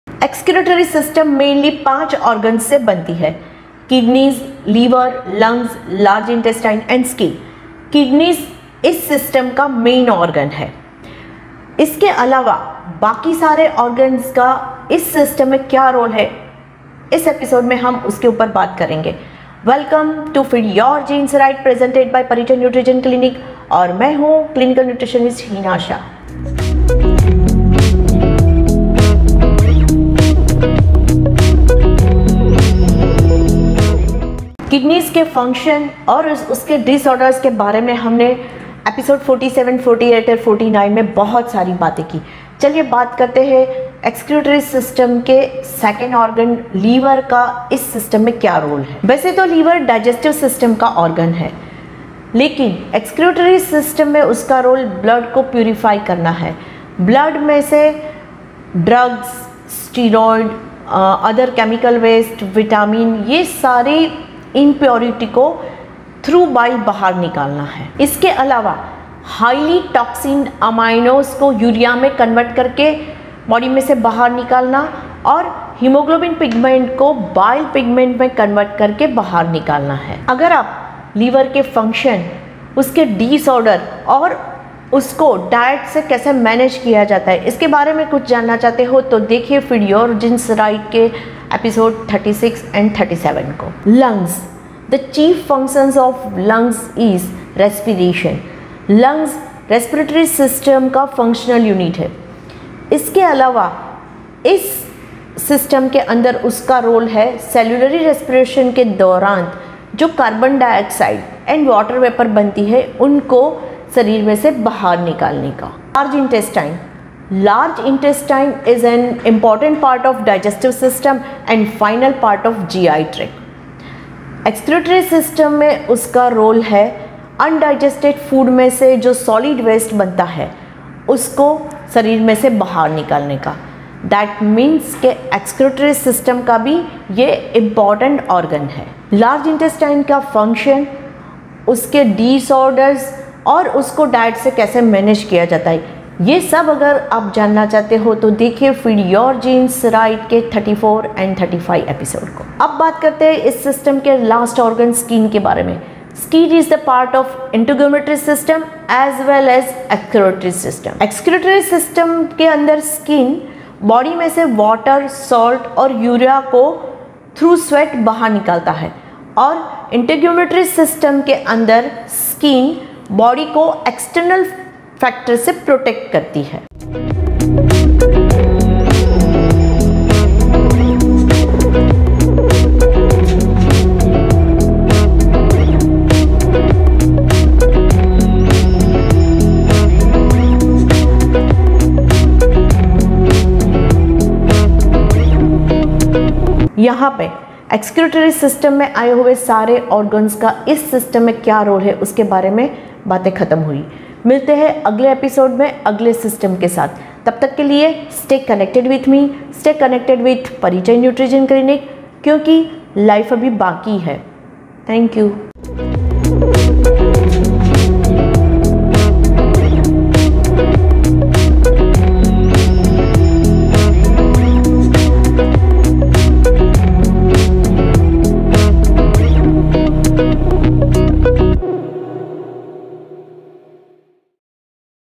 How Does The Human Excretory System Work - Kidneys, Liver, Lungs, Large Intestine, Skin
Hello, I am Heena Shah, a clinical nutritionist at Nutri-Gene clinic.

In today’s video, we will talk about extractor system and functional organs for the system.

The excretory system is the part of an organism's body that is responsible for biological process of removing waste from your body.

The excretory system is in charge of removing wastes generated by homeostasis.

The liver, lungs, large intestine, kidney and skin are among the body organs that are engaged in this process.

Each organ works differently and has its own importance for running the body efficiently.

Watch the video to know the functions of these five organs in excretory process.